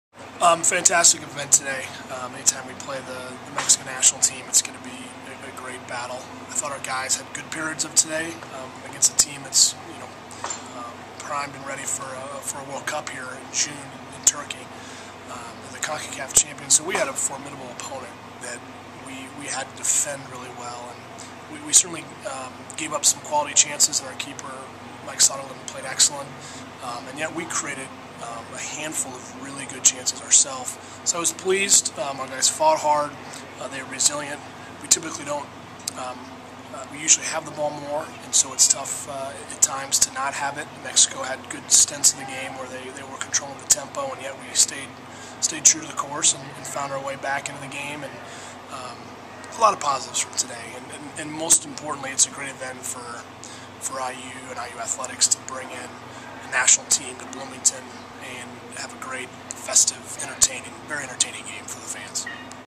IU vs Mexico Recap
Head Coach Todd Yeagley recaps Indiana's 1-0 loss to the U-20 Mexican National Team.